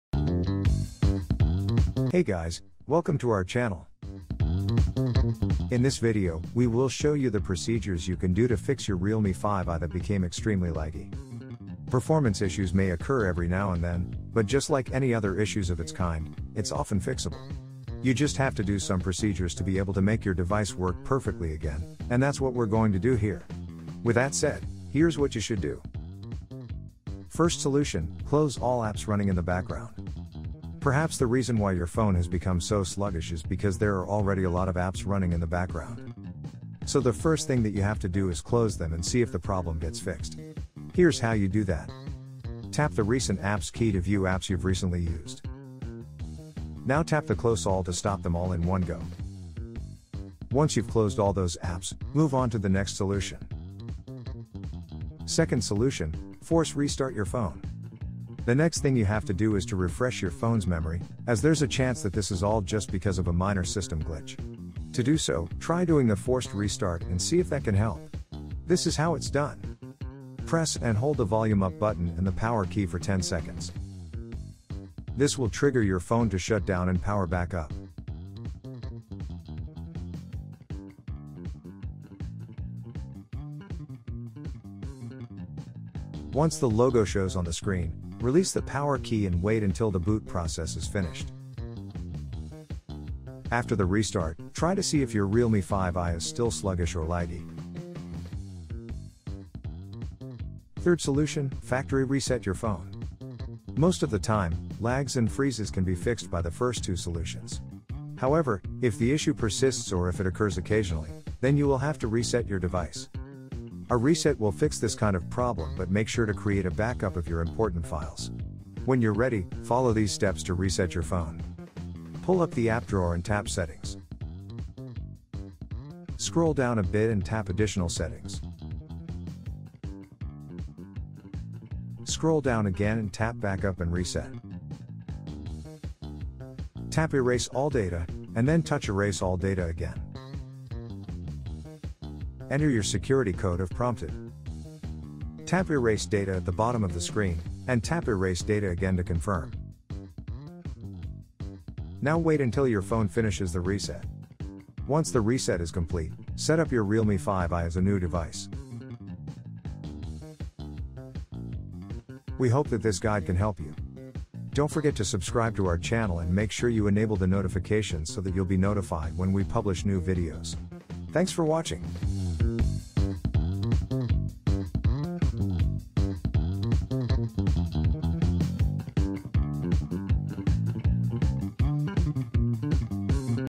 How To Fix A RealMe 5i That’s Extremely Laggy
In this video, we will show you the procedures you can do to fix your RealMe 5i that became extremely laggy.

Performance issues may occur every now and then, but just like any other issues of its kind, it’s often fixable. You just have to do some procedures to be able to make your device work perfectly again, and that’s what we’re going to do here. With that said, here’s what you should do:

00:00 Introduction
00:25 First solution: Close all apps running in the background
00:56 Second solution: Force restart your phone
01:41 Third solution: Factory reset your phone

We hope that this guide can help you.